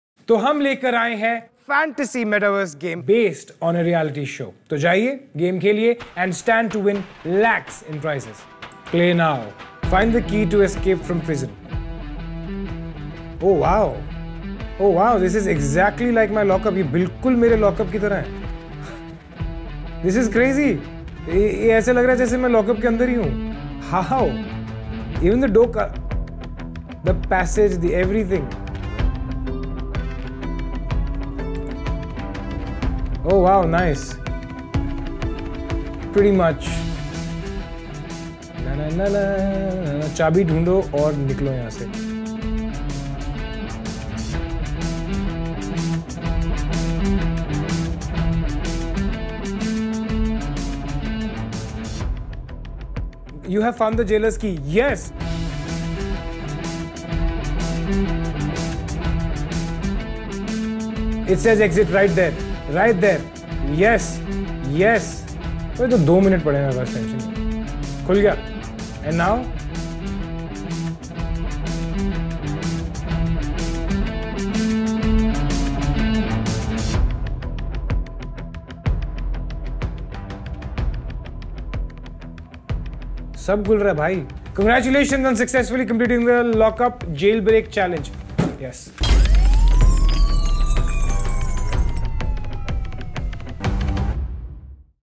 Lockupp Game | Karan Kundra | ALT Balaji | MX Player
The LockUpp jailor Karan Kundrra is waiting for you to play the LockUpp metaverse game.

The LockUppGame is officially live, go play the Jailbreak challenge now

and start playing to win prizes worth lacs.